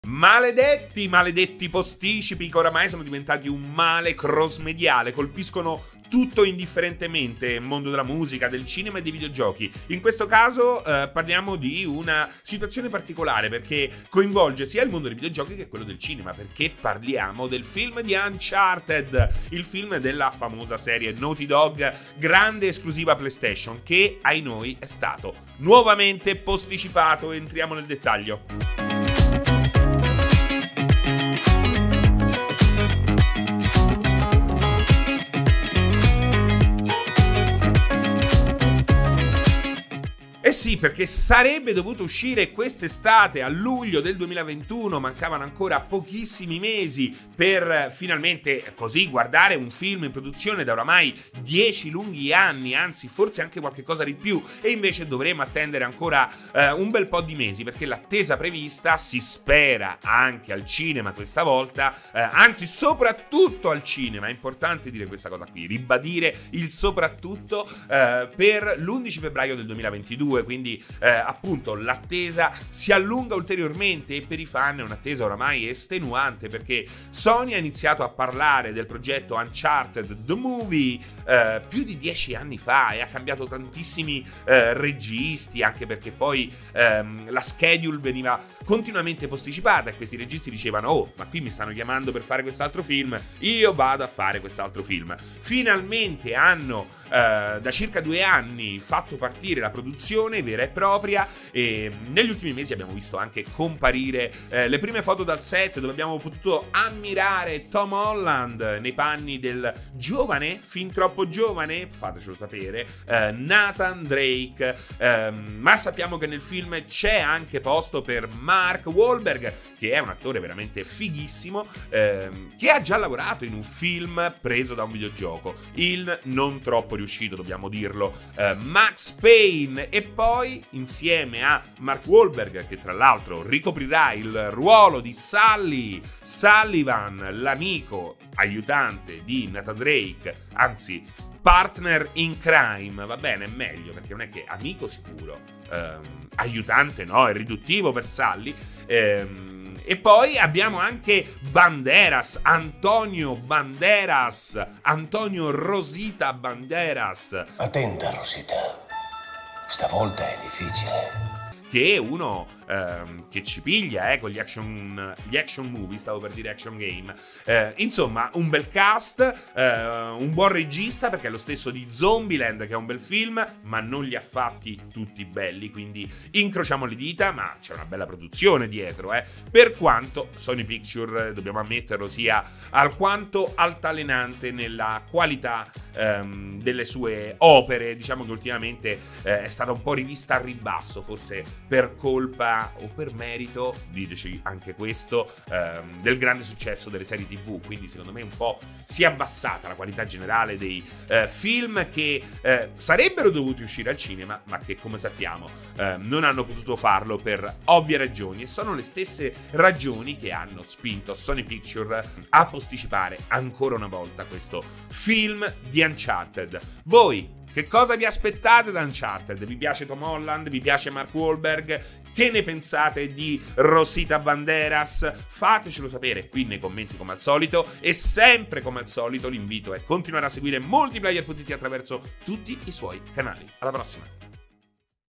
Uncharted il Film, nuovo rinvio: quando uscirà?
Il film di Uncharted è stato rinviato per l'ennesima volta, come riportato da Deadline. Uscirà nel 2022, tra più di un anno, invece che il 16 luglio 2021 come da precedente annuncio.